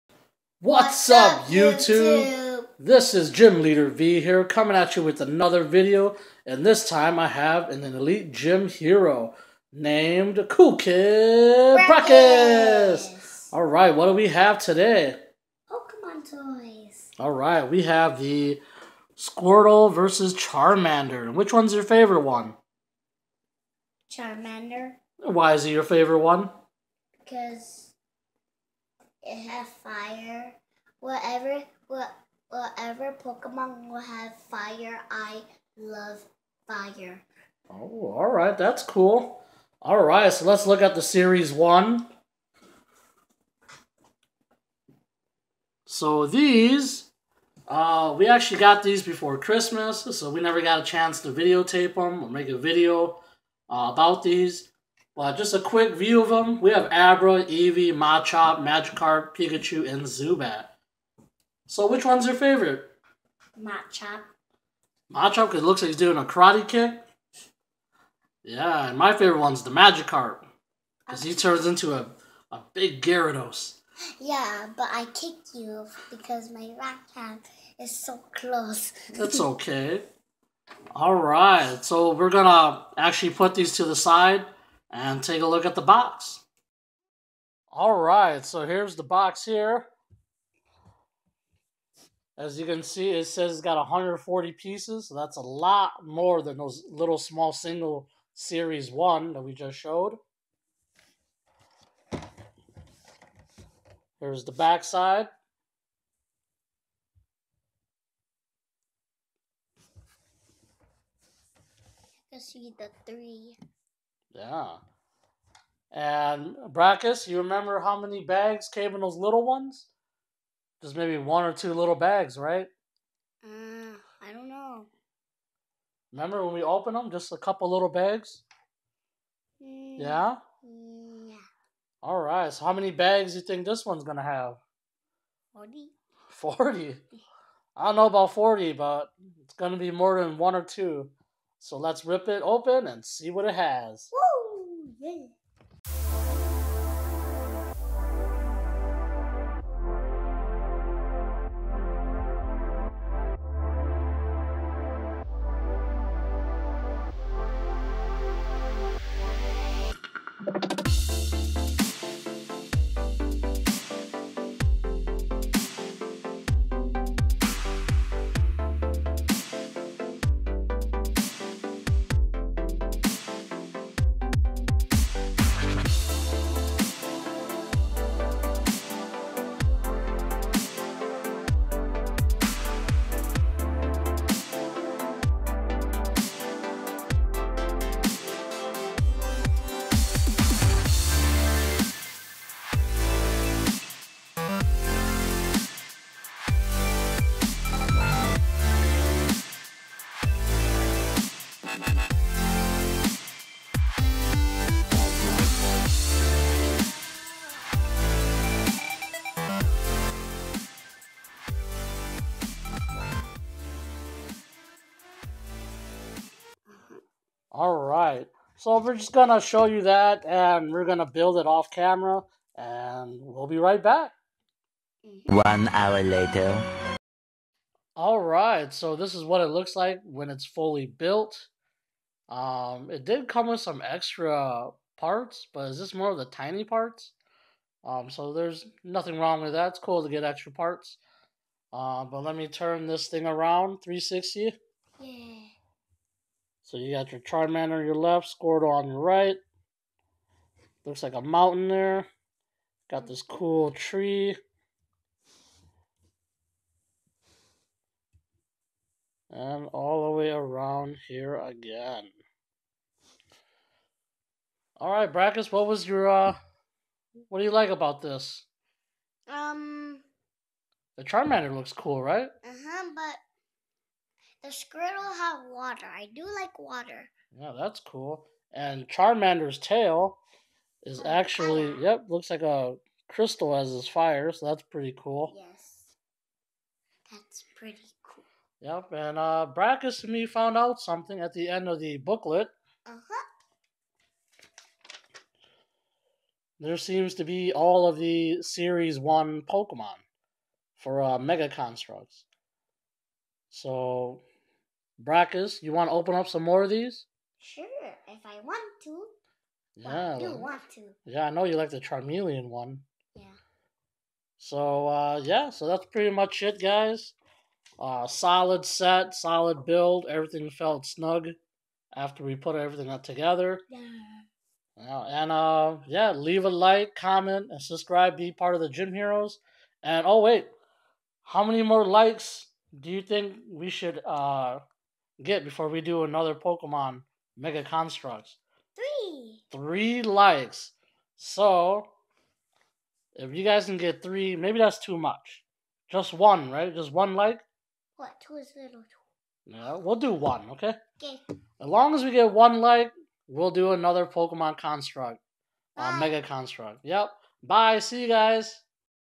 Unboxing Mega Construx Pokemon: Squirtle vs Charmander
I will be introducing our 1st Elite Gym Hero Cool Kid Brakkus in this video.  He will also be helping me unbox the squirtle vs chamander set from mega construx pokemon series.  Let's get this video to 1 like so we can continue to bring you more mega construx pokemon.